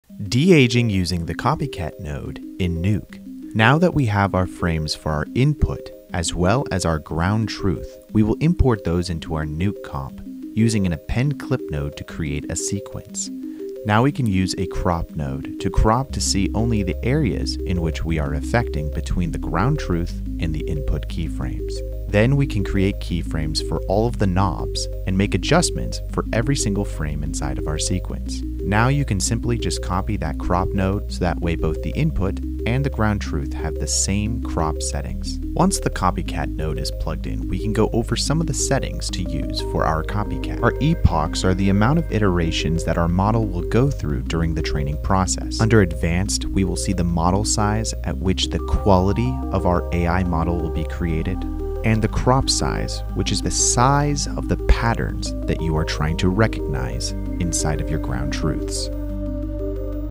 DeAge with @ConnorThomas - Part 2
Part 2 of our de-aging series using Nuke CopyCat with @ConnorThomas  shows you how to use different nodes between your Input and Ground Truth.

About Us:
Foundry creates award-winning creative software so global studios can turn incredible ideas into reality.